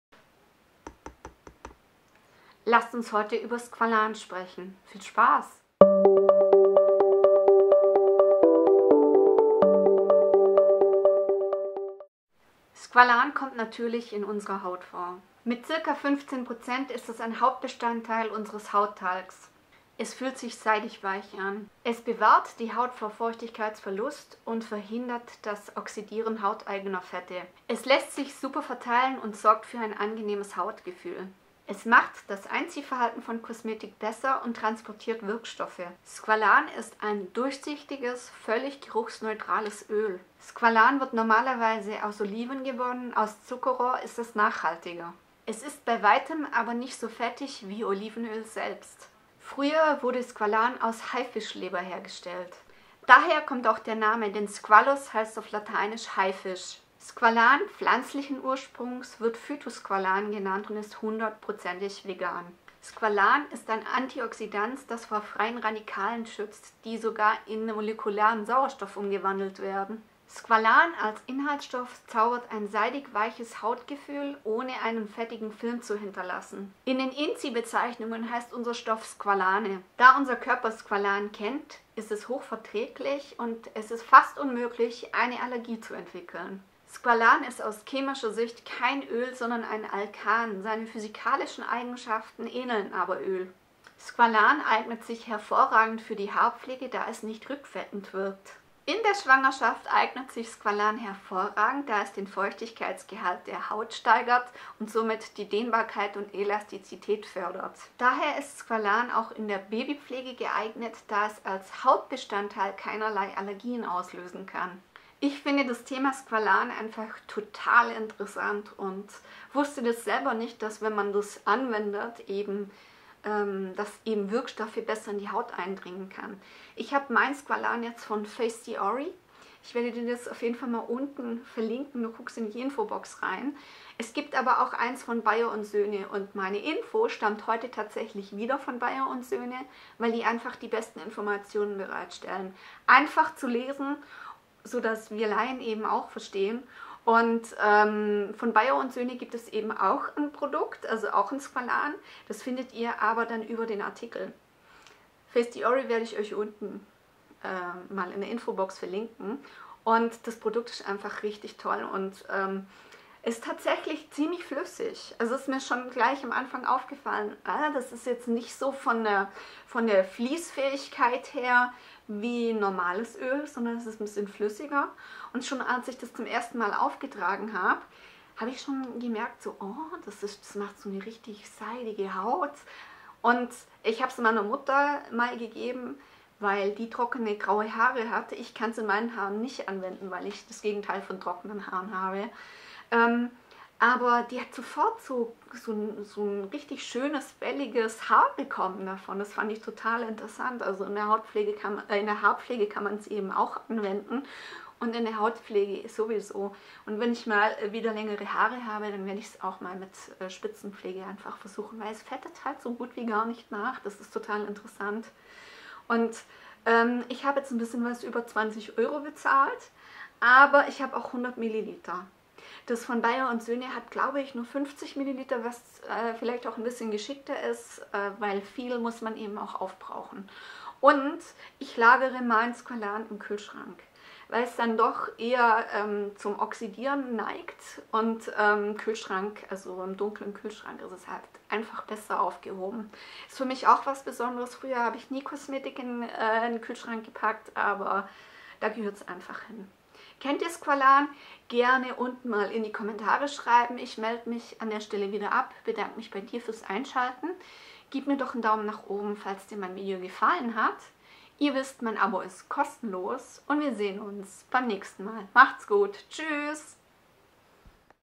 Squalan bewahrt den Feuchtigkeitsverlust der Haut - 20 Fakten
Meine ersten Erfahrungen mit Squalan habe ich mit diesem Fläschchen. Gehört habe ich es aber schon länger und das es so toll verträglich ist.
1. Spualan kommt natürlich in unserer Haut vor.
2. Mit circa 15% ist es ein Hauptbestandteil des Hauttalgs.
3. Es fühlt sich seidig weich an.

WERBUNG
Als Amazon Partner verdiene ich an qualifizierten Käufen
ZERO WASTE